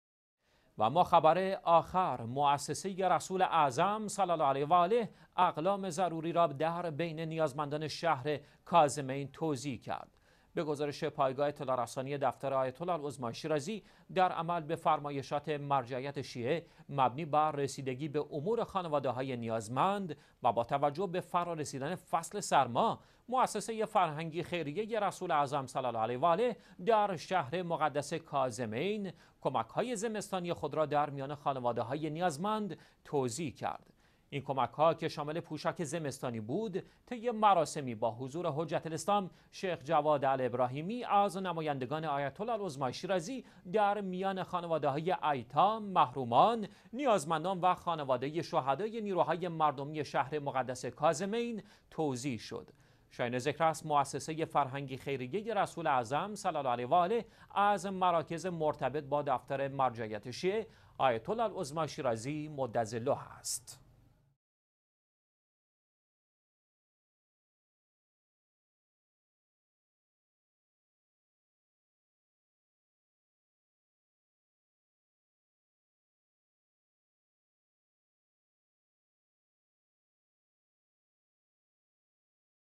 توزیع کمک های زمستانی در میان خانواده های نیازمند شهر کاظمین
موسسه_رسول_اعظم صلی الله علیه و آله، اقلام مورد احتیاج نیازمندان را در بین آنها توزیع کرد.

به گزارش پایگاه اطلاع رسانی دفتر آیت_الله_العظمی_شیرازی، با فرا رسیدن فصل سرما و در عمل به فرمایشات مرجعیت شیعه مبنی بر رسیدگی به امور خانواده های نیازمند، مؤسسه فرهنگی ـ خیریه رسول اعظم صلی الله علیه وآله، در شهر مقدس کاظمین، کمک های زمستانی خود را در میان خانواده های نیازمند توزیع کرد.

این کمک ها که شامل پوشاک زمستانی بود، طی مراسمی با حضور حجت الاسلام شیخ جواد ابراهیمی، از نمایندگان آیت الله العظمی شیرازی مدظله، در میان خانواده های ایتام، محرومان، نیازمندان و خانواده شهدای نیروهای مردمی شهر مقدس کاظمین توزیع شد.

شایان ذکر است، مؤسسه فرهنگی ـ خیریه رسول اعظم صلی الله علیه وآله، از مراکز مرتبط با دفتر مرجعیت شیعه، آیت الله العظمی سید صادق شیرازی مدظله، می باشد.

پنجشنبه - ۲۳ آذر ۱۳۹۶
الخمیس - ۲٥ ربيع الاول ۱٤۳۹